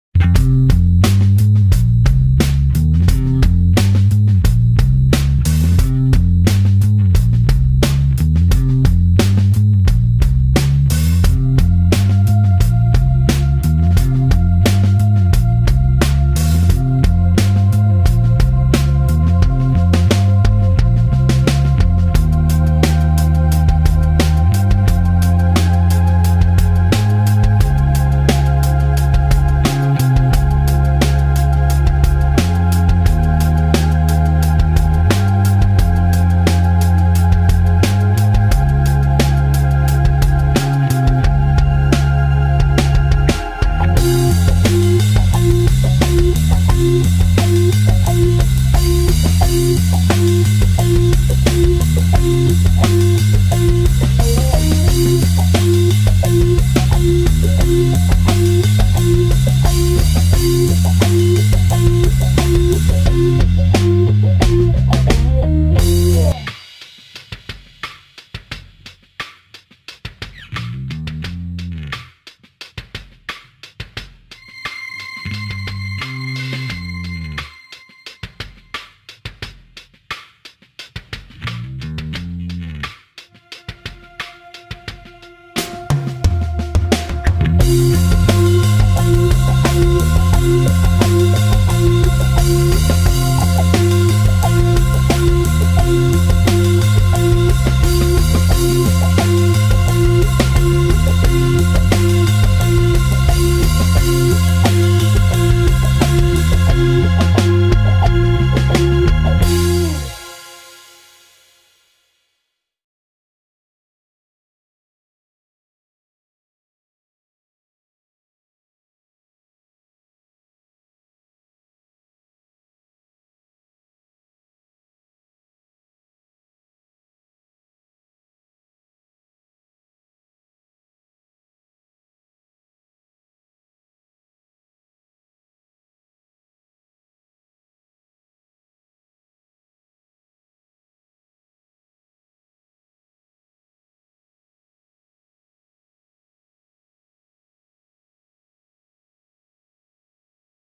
Automatic bus washing machine
Nissan clean India Pvt limited is proud introduce it's Automatic Car washing Machines Proven Technology Having In-depth Knowledge and experience in Car wash Segment Our Machine washes Thousand of car Accros India